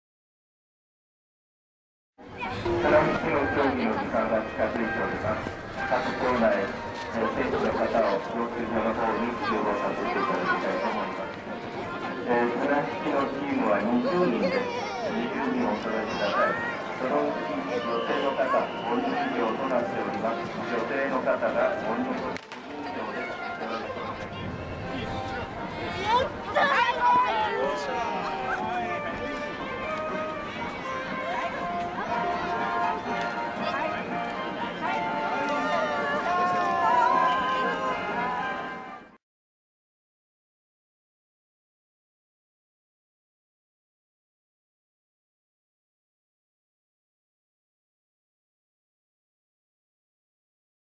二人三脚
震災復興二年目、元気で恒例平成２１年度「比角大運動会」が盛大におこなわれた